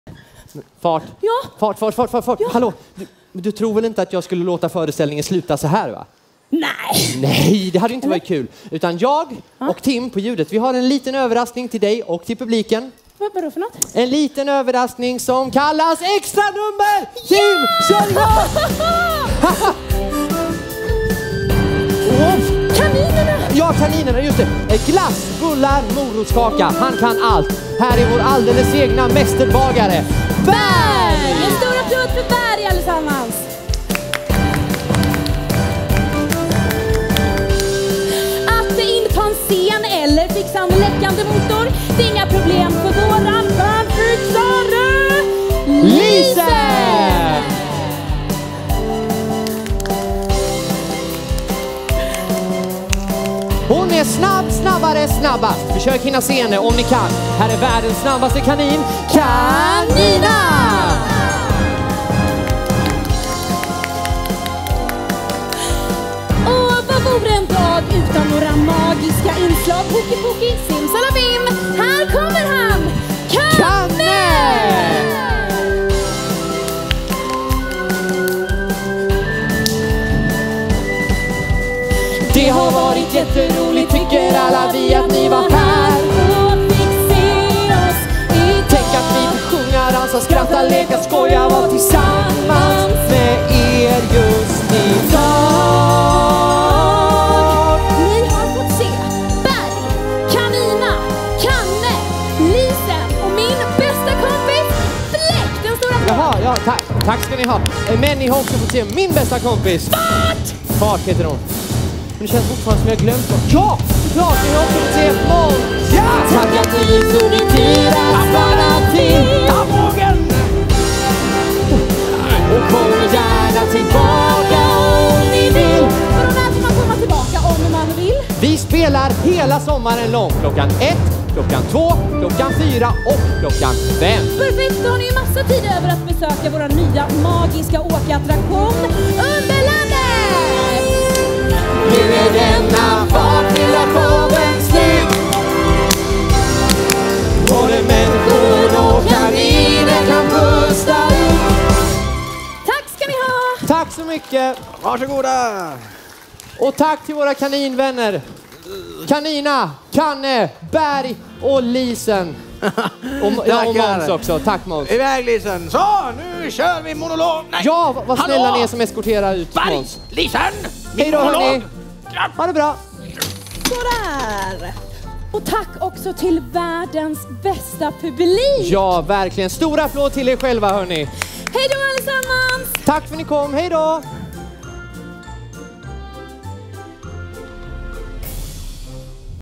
Dur & Retur – Extranummer (Kaninteater sommar 2021)
Dur & Retur – en musikalisk resa som går som på räls. Extranummer från föreställningen på Kaninscenen under sommarsäsongen på Liseberg 2021.

Kaninernas tokiga shower och föreställningar blir ännu roligare på plats vid Kaninscenen. Kom förbi nästa gång du besöker Kaninlandet på Liseberg!